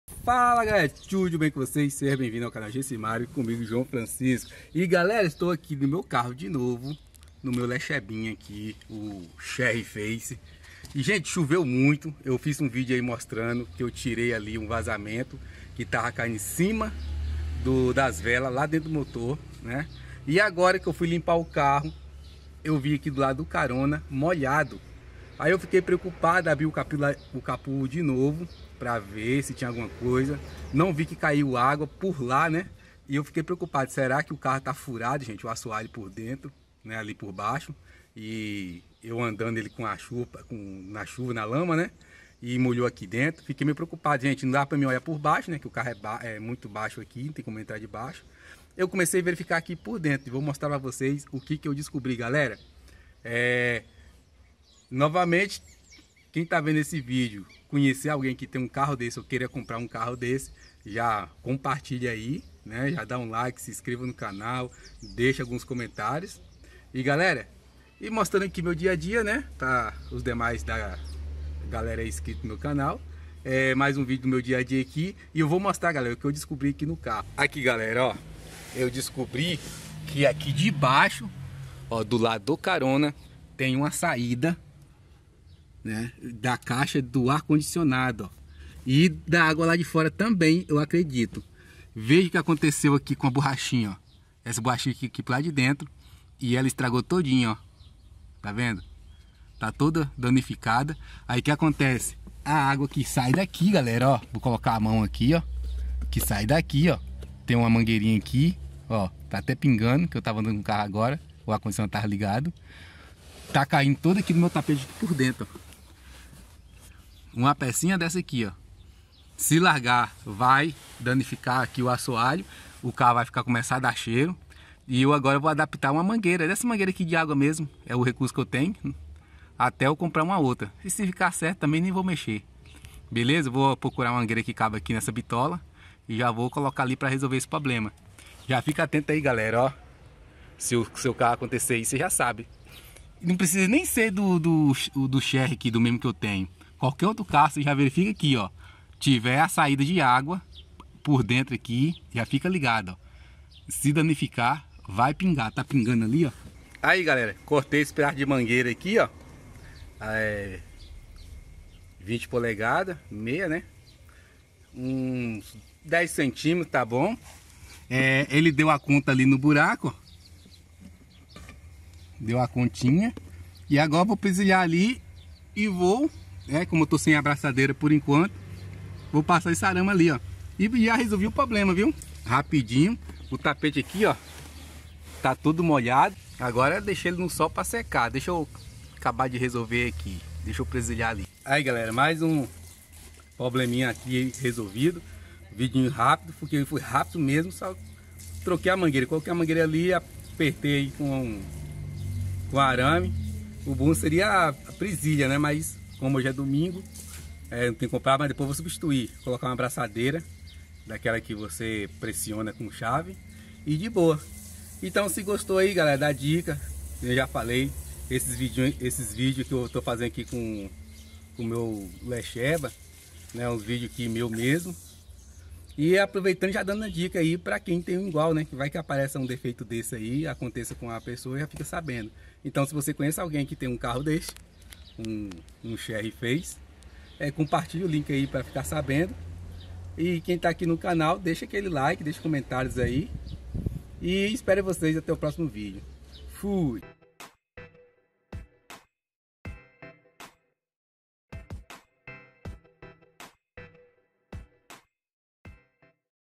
Entrou água dentro do lexeba o chery face ainda bem que descobri a tempo
salve galera hoje limpando o meu lexeba descobri um vazamento de água dentro do carro  lado do passageiro  proveniente do ar condicionado ,ainda bem que descobri a tempo e já resolvi o problema
Seja membro deste canal e ganhe benefícios: